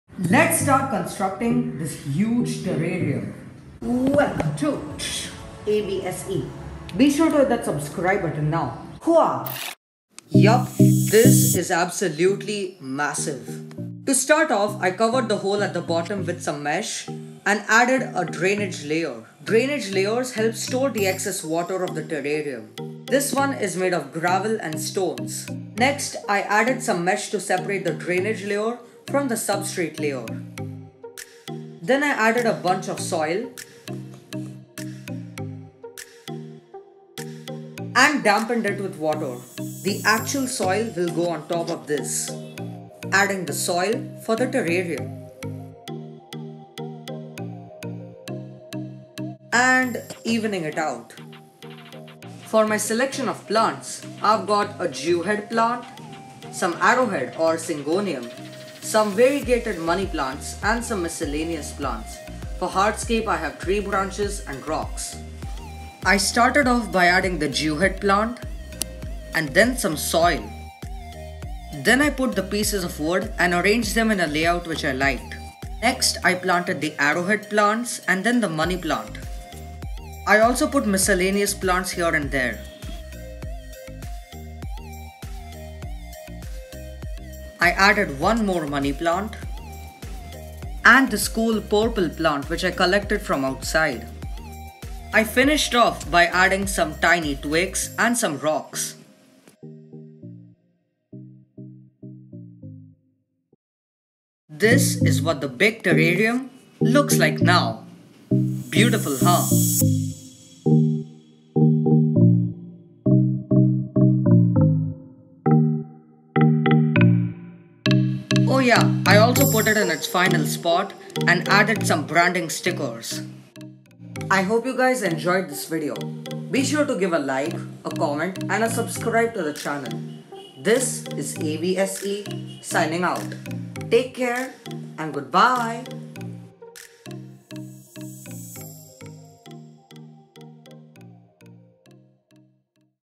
So I made a HUGE TERRARIUM... (A.B.S.E.)
I made a HUGE terrarium! No creatures reside here, just plants...
I put a ton of plants inside, and it's really heavy (well over 7 kilograms!) !
The species of plants are listed below:

Golden Pothos (Variegated Moneyplant)
Arrowhead Plants (Syngonium sp.)
Jewhead Plant (Tradescantia zebrina)
Other miscellaneous plant species

Today on A.B.S.E., inside the Creatureverse, I shall construct a HUGE terrarium- with a lot of plants, soil and wood pieces! This thing weighs well over 7 kilograms, and looks absolutely BEAUTIFUL! Watch this video till the end to find out more!!!

BigBuild Creatureverse A.B.S.E.

Season 2